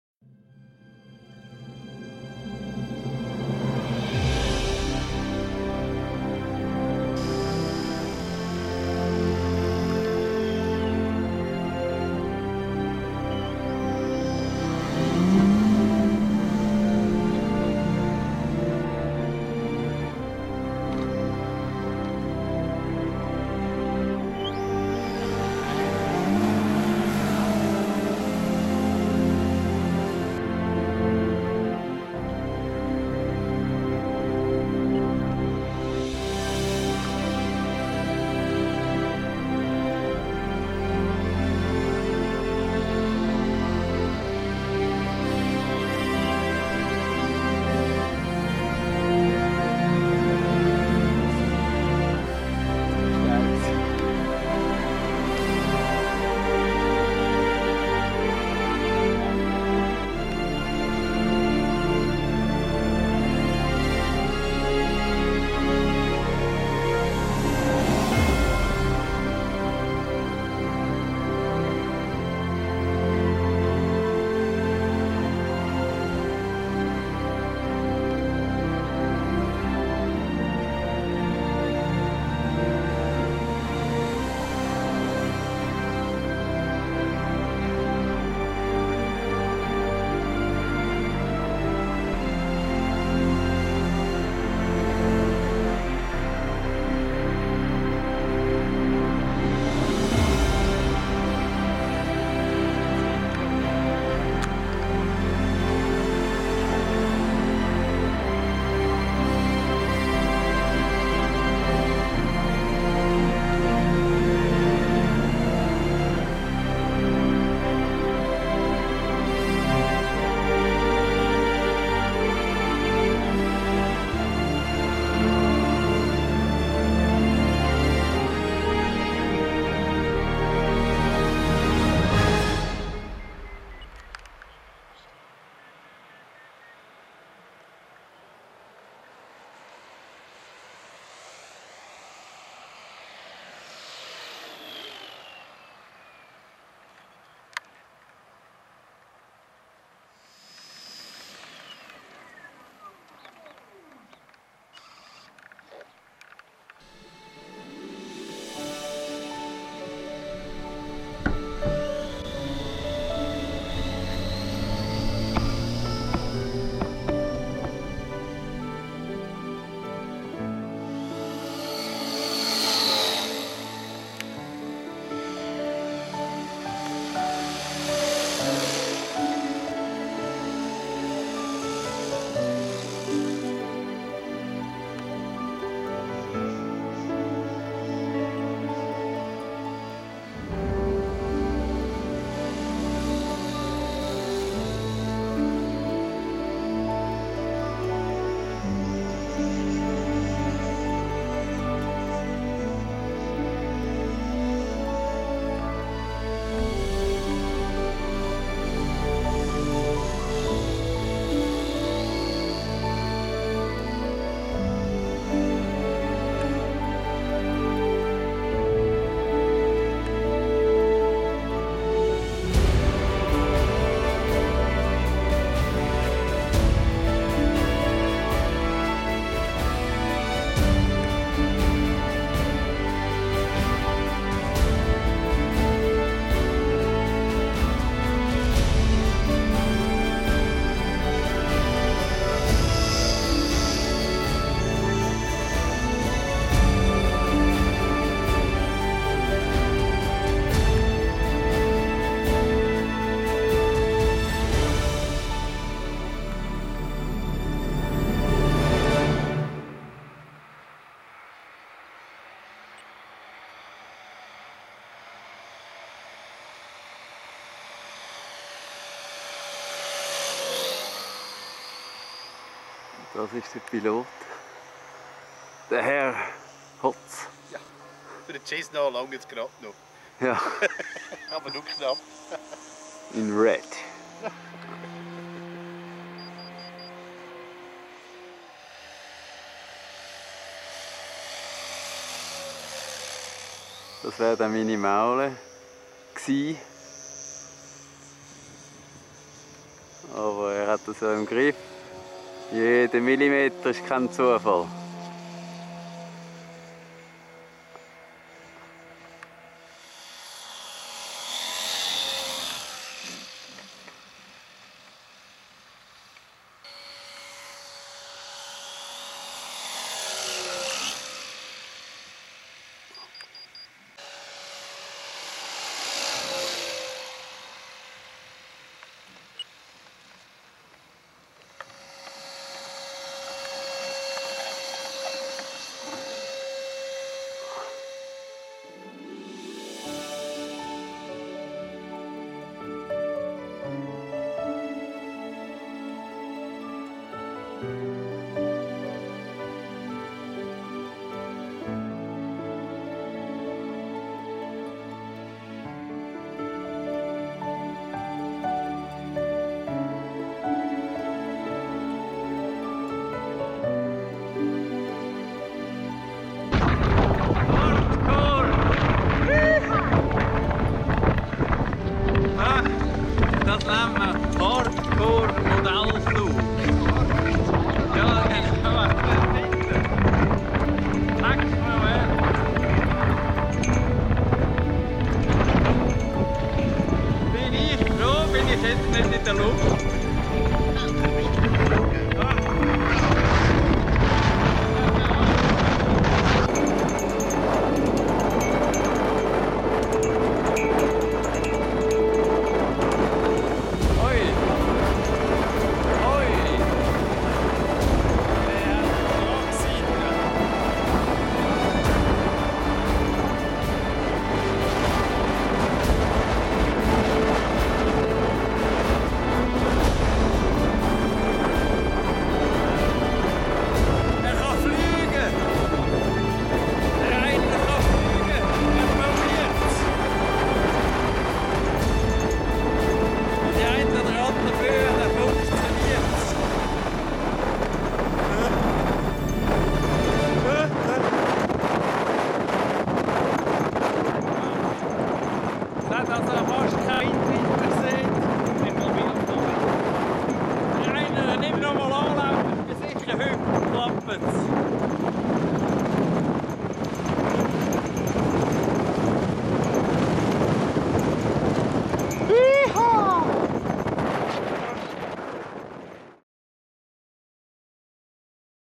MG Buttikon mit einem stürmischen Ende
Im Video sind zu sehen Cessna 182 , Maule, Mü 28